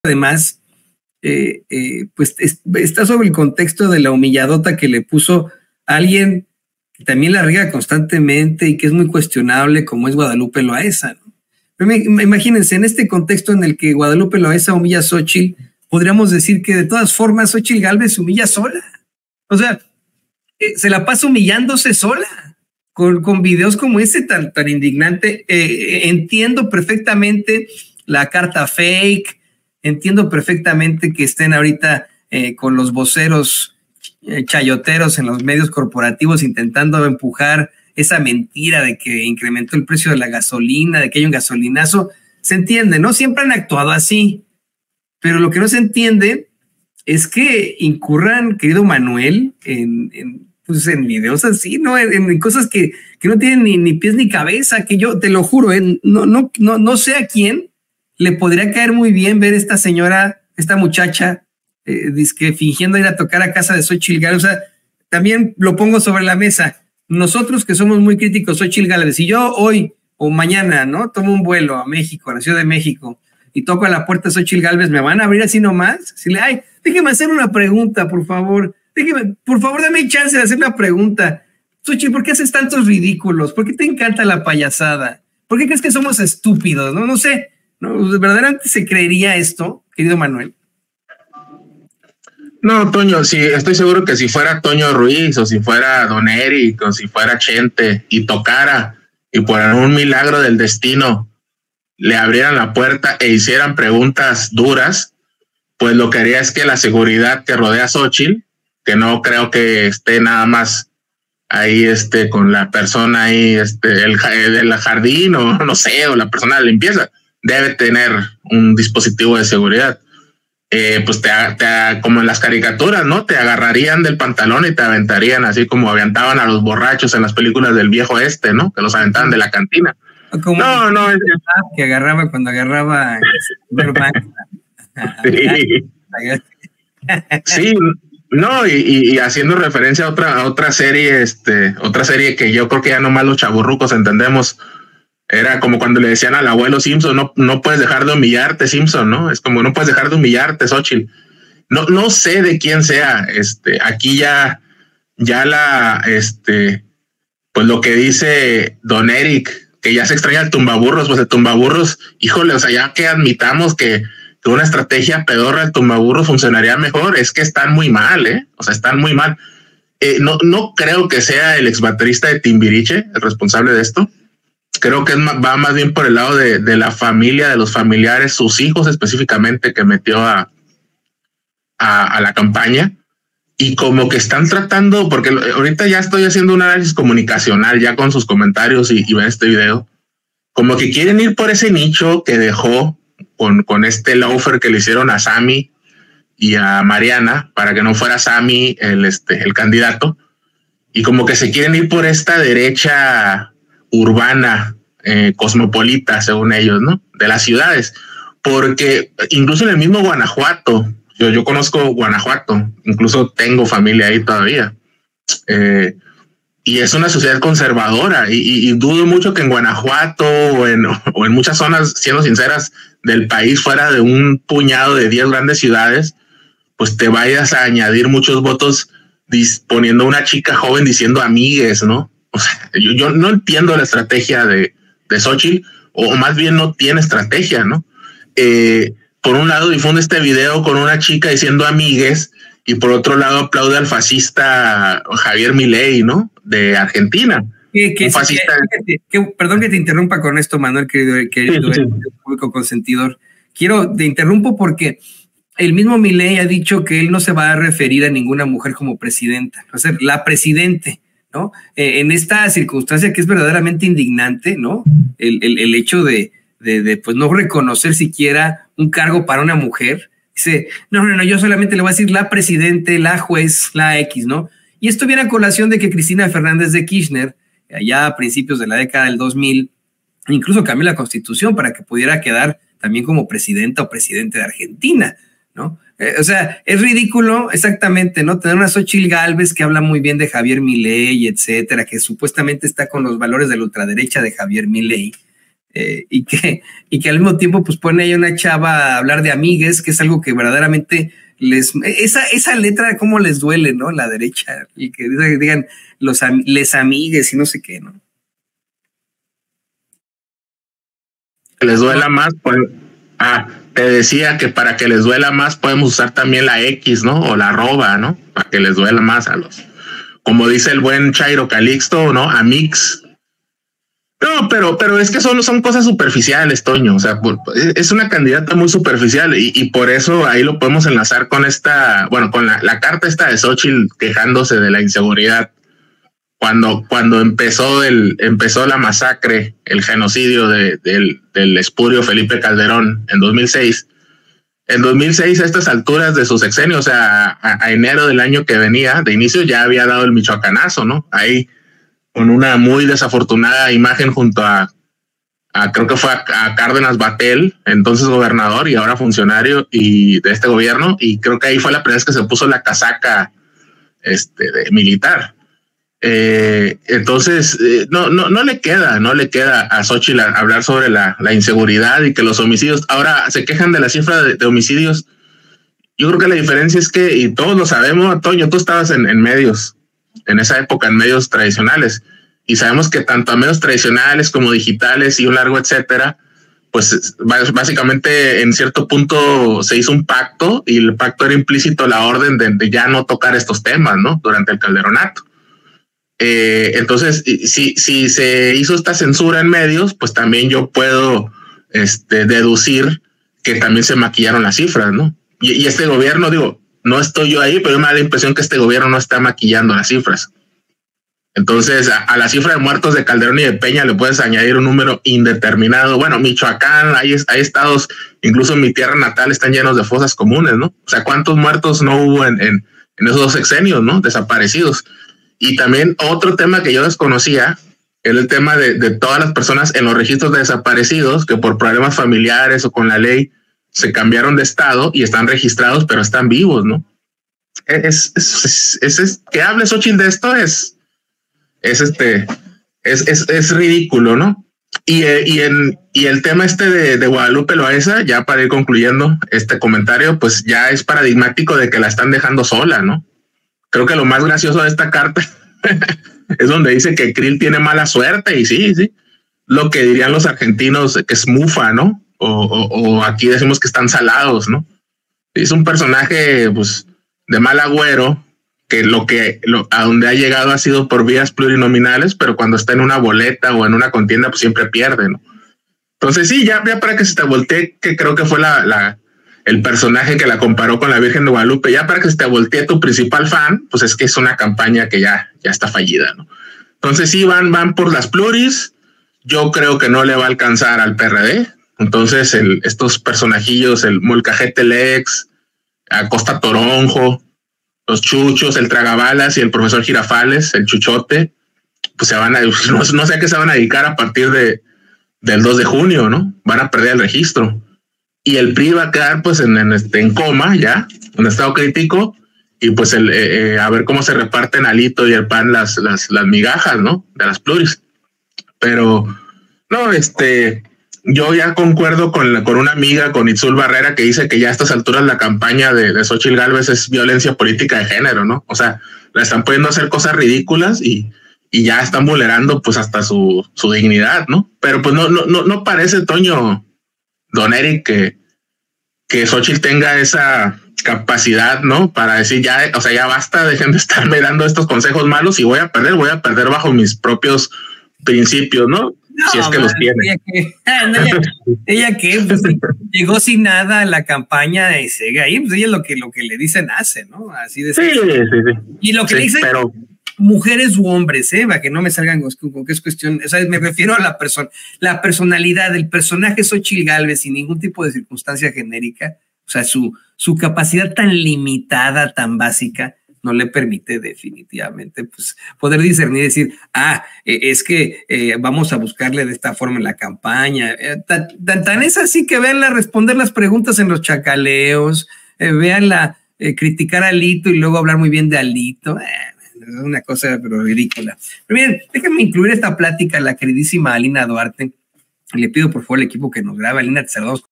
CON QUÉ CARA XÓCHITL HABLA DE INSEGURIDAD, DICE MANUEL IBARRA TRAS CARTA DE LA PANISTA A AMLO
Sobre ese video chafa de la joven yendo a tocar la puerta de la casa de Xóchitl Gálvez para hacerle una pregunta, el periodista independiente y “Martillo de los Fachos” Manuel Ibarra dijo que si fuera un periodista de verdad, la seguridad que rodea a Xóchitl Gálvez, lo agarrarían cual borracho del viejo oeste. Y de la carta de Xóchitl reclamándole al presidente López Obrador, simplemente dice que a la panista nomás no le queda hablar de inseguridad y calla sobre el sexenio de Borolas cuando este le declaró la gu3rr4 al n4rc0